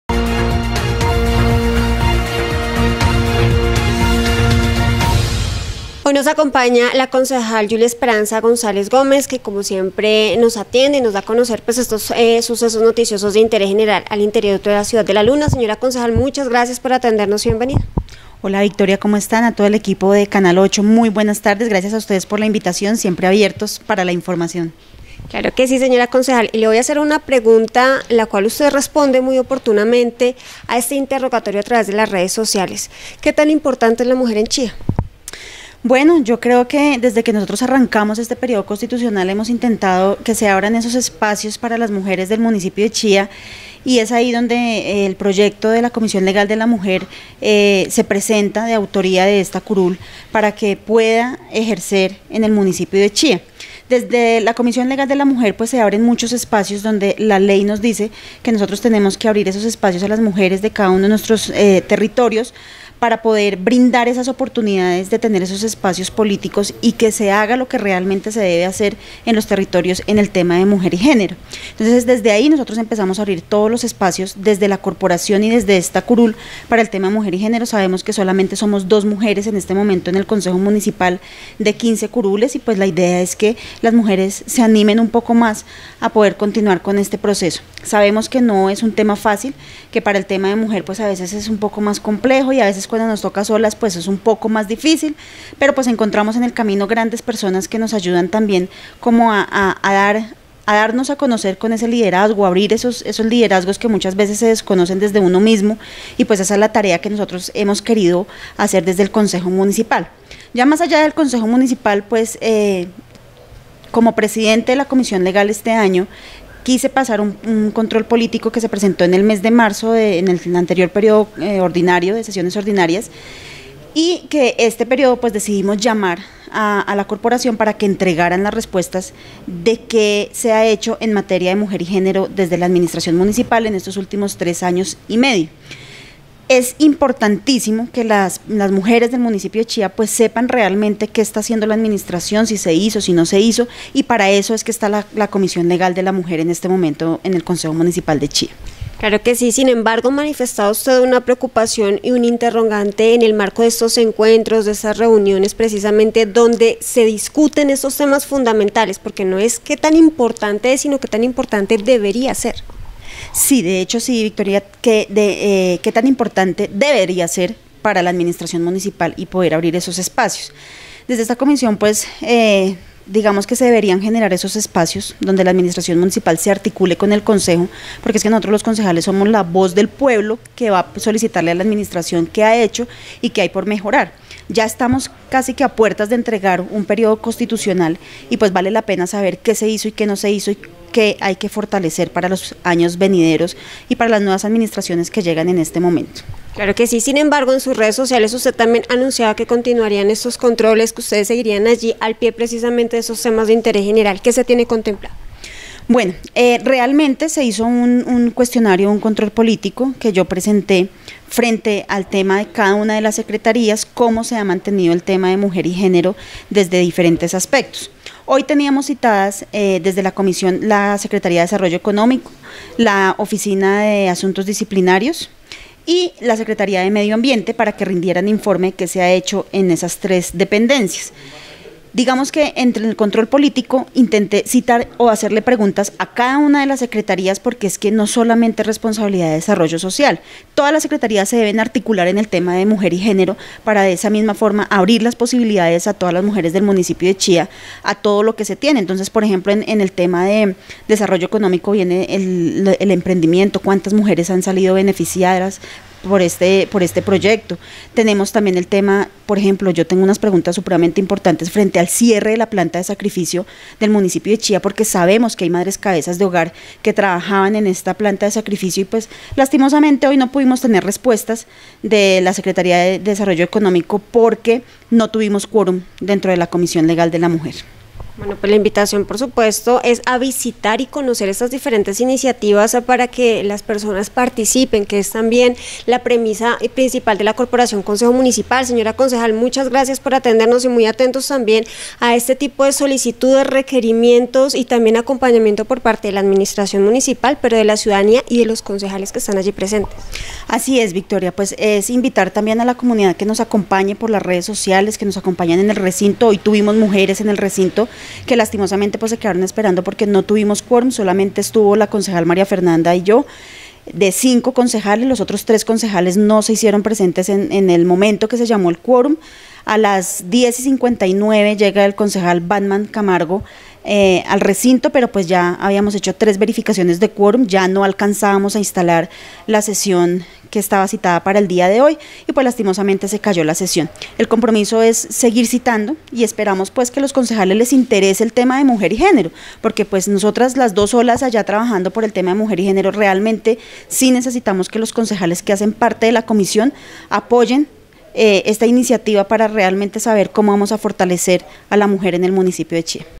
La concejal de Chía Yuli González, habló para las cámaras de canal 8
Se refirió a varios temas de interés general, así mismo habla sobre la importancia de la mujer en Chía y porque se llevarán cabo diferentes encuentros en el marco de este importante tema.